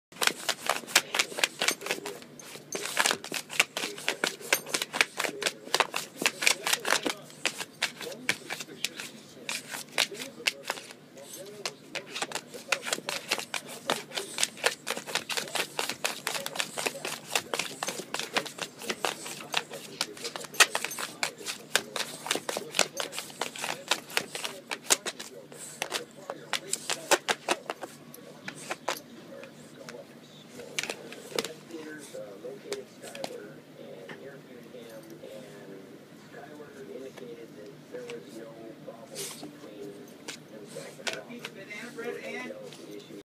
Brutus the BullMastiff Pup (14wks)
Helping with laundry.